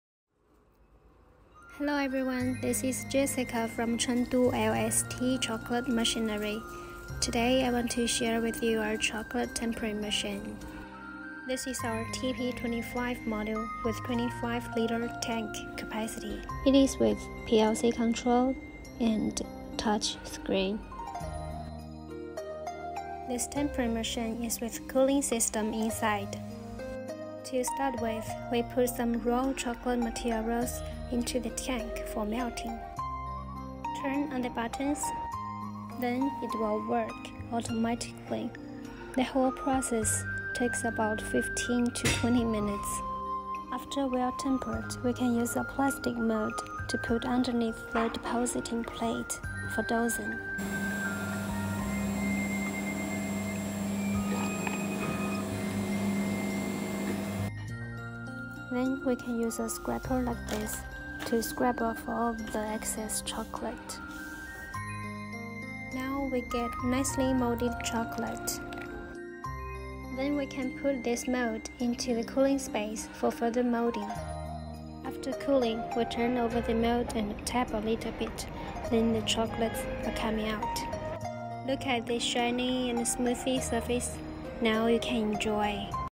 How to use the chocolate tempering machine?
Tempering is the essential step in chocolate making process, well tempered chocolate will have better taste and longer shelf life. The are many ways to temper chocolate, but It is very troublesome for manual tempering. So tempering machines are very important for bulk production.